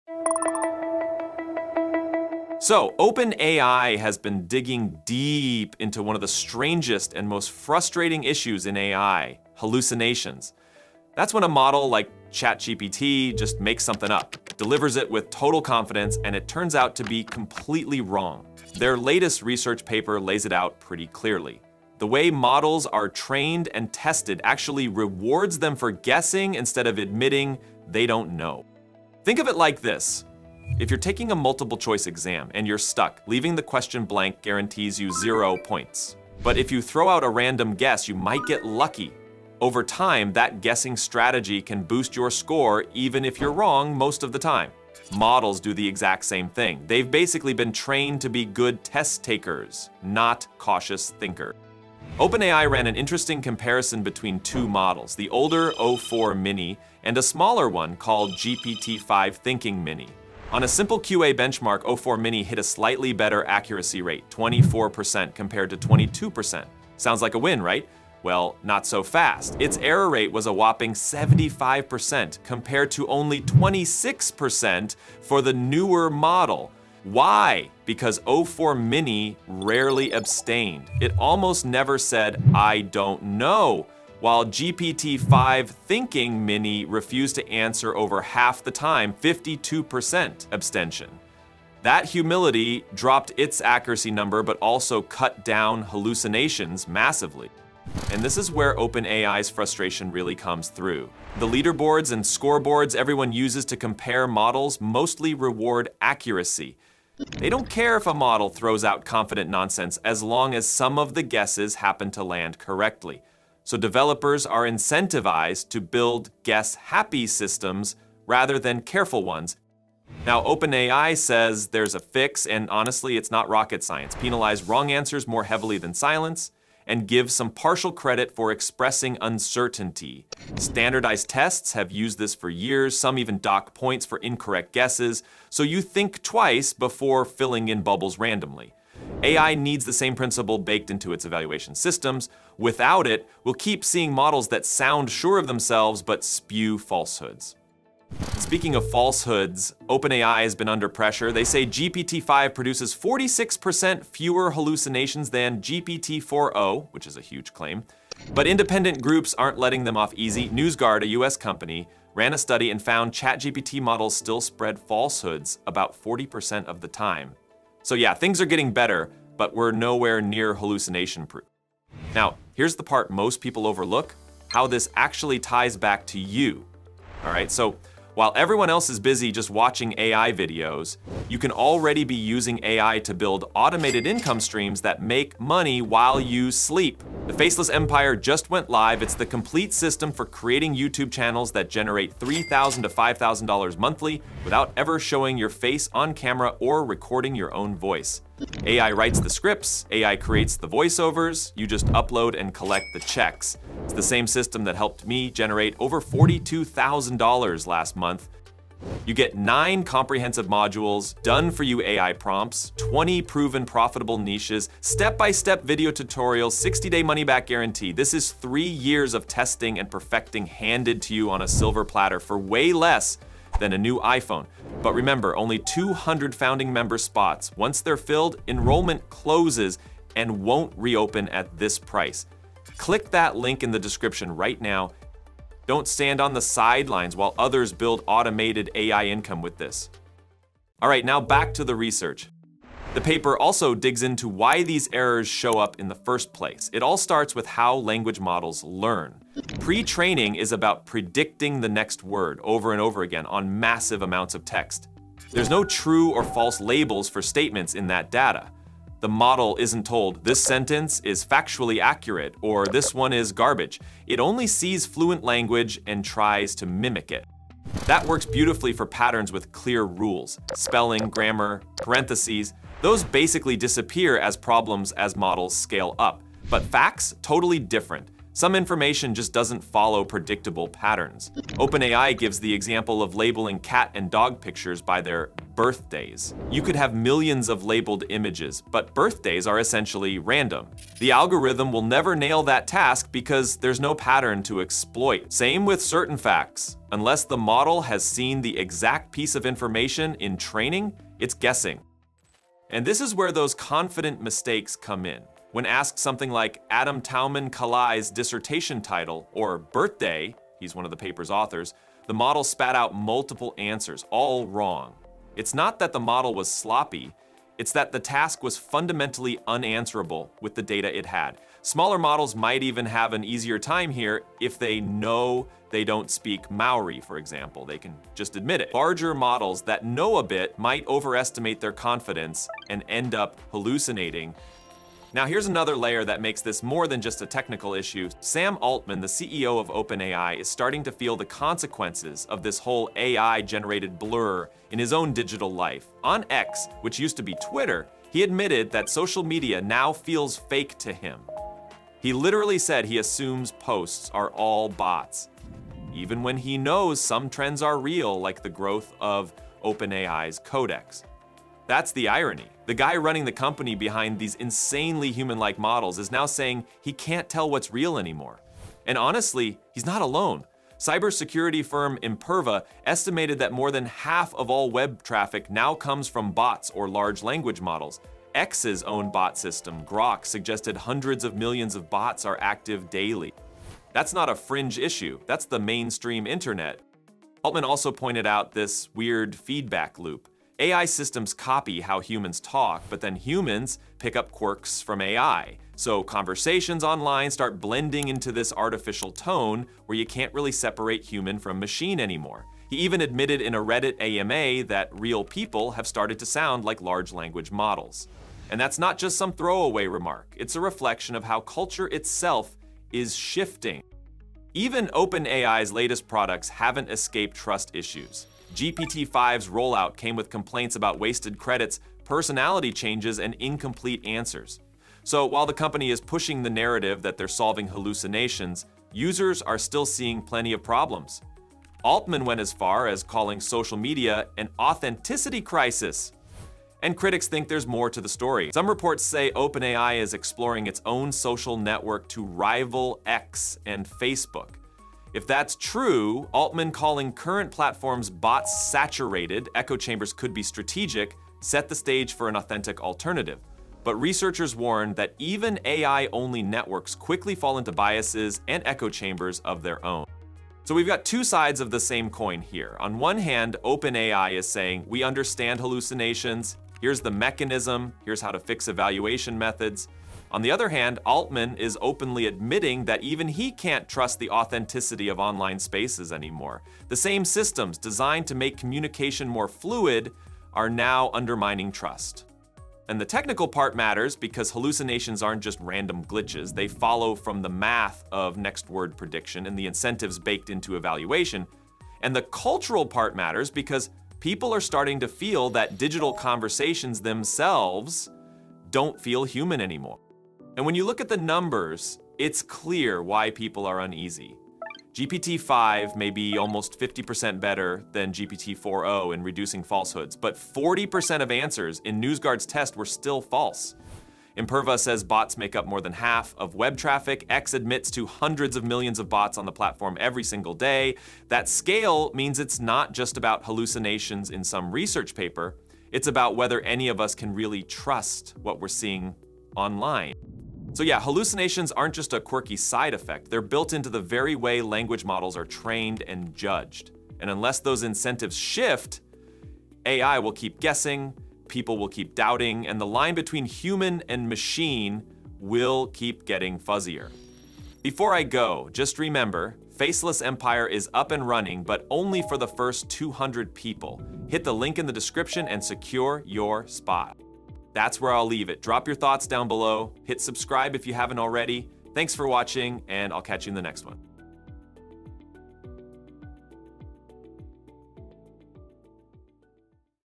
OpenAI Just Exposed GPT-5 Lies More Than You Think, But Can Be Fixed
OpenAI just revealed why AI models keep lying with total confidence — and the fix they say can finally stop it. Their new research shows that current benchmarks actually reward guessing instead of caution, which is why models hallucinate so often. GPT-5 may cut hallucinations by almost half compared to GPT-4o, but tests still show falsehoods in 40% of answers. Even Sam Altman admits the internet now feels fake, with bots making up more than half of all web traffic. From inflated leaderboards to a rising authenticity crisis online, this story shows the real reason we can’t fully trust AI yet.

🦾 What You’ll See:
• OpenAI’s research on hallucinations and why AI keeps lying
• GPT-5 vs GPT-4o on accuracy and falsehood rates
• Why benchmarks reward confident nonsense instead of caution
• Sam Altman admitting the internet feels fake and bot-saturated
• Independent studies showing ChatGPT still spreads falsehoods 40% of the time
• Why bots now dominate over half of web traffic
• The cultural shift where humans start sounding like AI
• How OpenAI plans to fix evaluation methods

⚡ Why It Matters:
This isn’t just about model glitches — it’s about trust, truth, and whether humans can still tell what’s real online. OpenAI’s research may point to a fix, but the fight against AI hallucinations has only just begun.